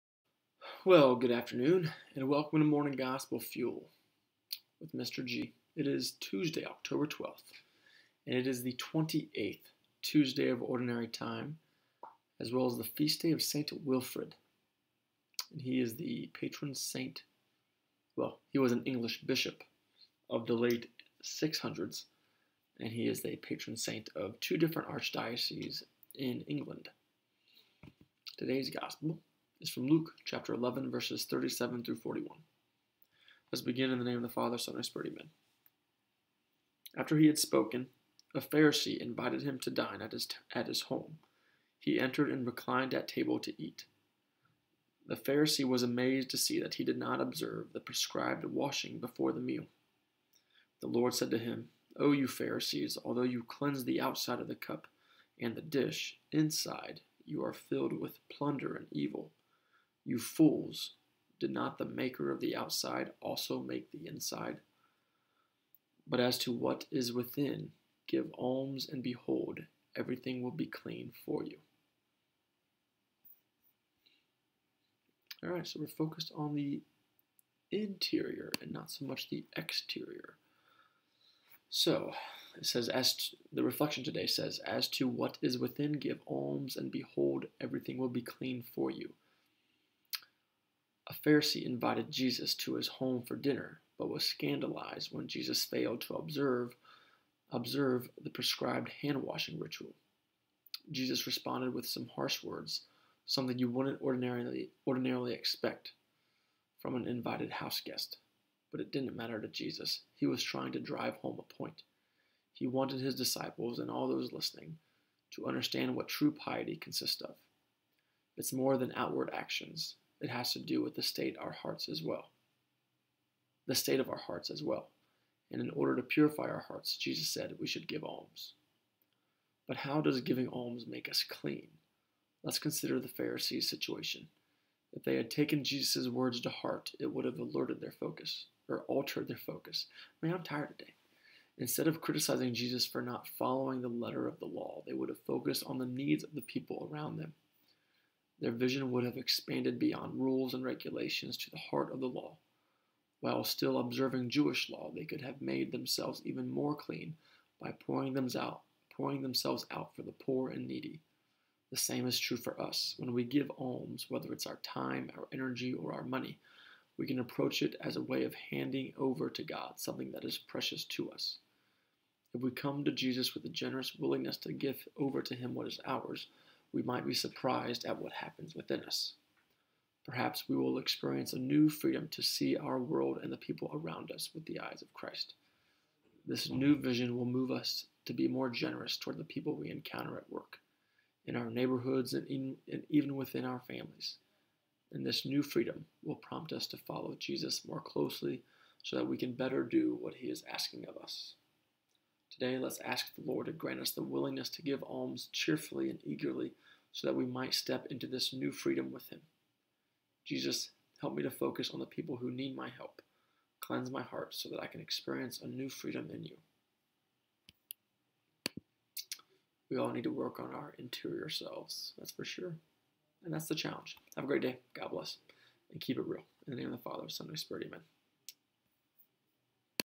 MGF with Mr. G (10-12-21: Denunciation of the Pharisees & Scholars of the Law) - 28th Tuesday of OT
Here is today's Tuesday Gospel (Year 1) of Ordinary Time 2021 with a brief reflection from a former high school math teacher and coach who finds great value in daily Scripture and time spent with the Lord. Today is the Feast Day of St. Wilfrid, and the Gospel reading is from Luke 11: 37-41.